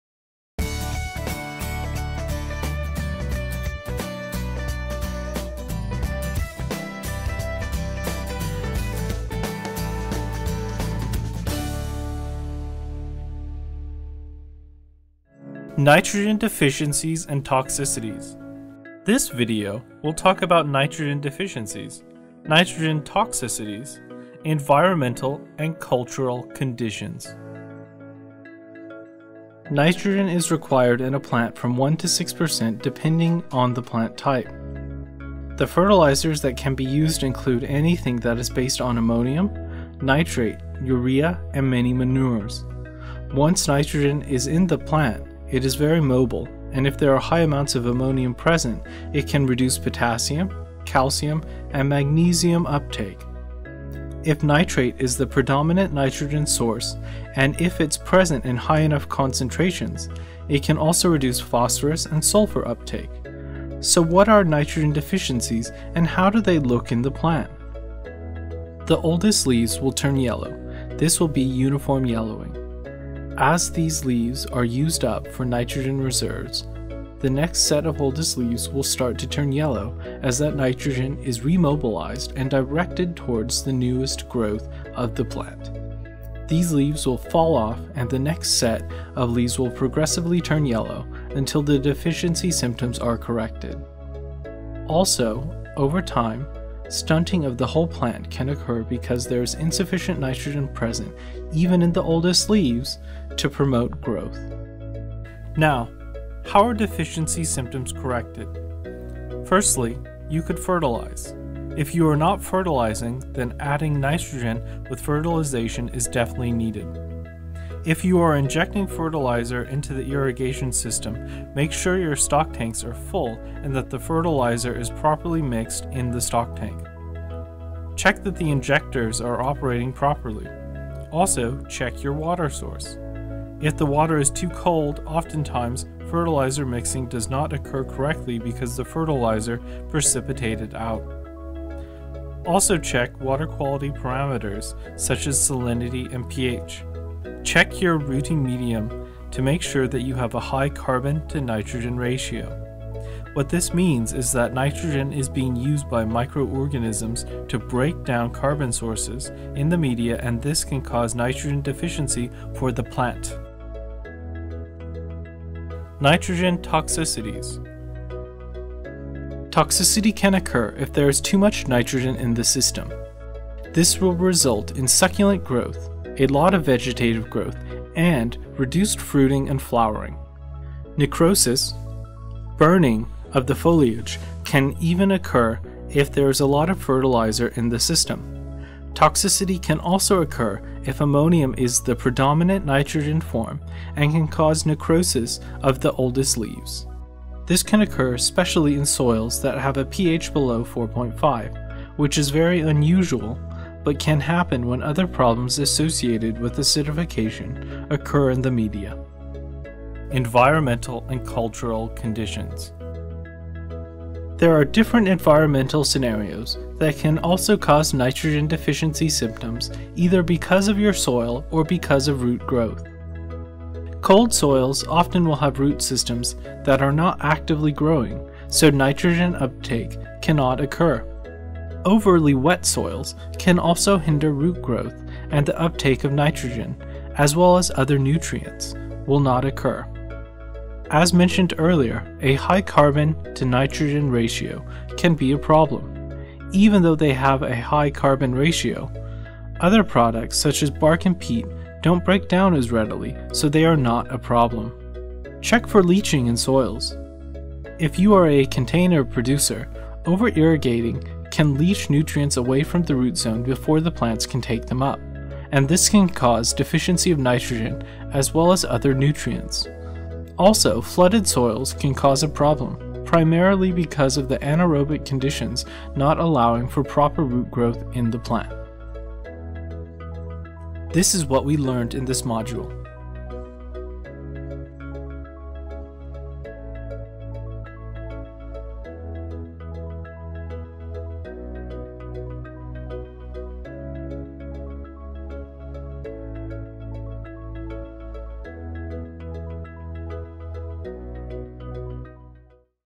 Nitrogen Deficiency and Toxicity Issues in Nursery Production
Learn how to identify and fix nitrogen deficiency and nitrogen toxicity problems in nursery production systems.

To see more information about University of California Nursery and Floriculture Alliance
The full list of videos on Fertilizers and Plant Nutrition in Nursery Production in English and Spanish are